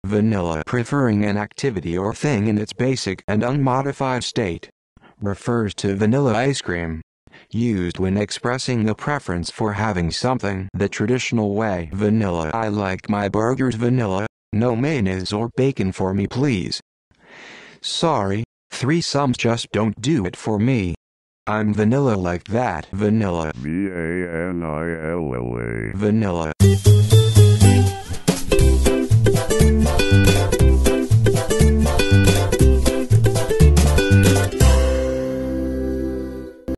Vanilla meaning and pronunciation
Preferring an activity or thing in its basic and unmodified state. Refers to vanilla ice cream. Used when expressing a preference for having something the traditional way.

Vanilla definition by Urban Dictionary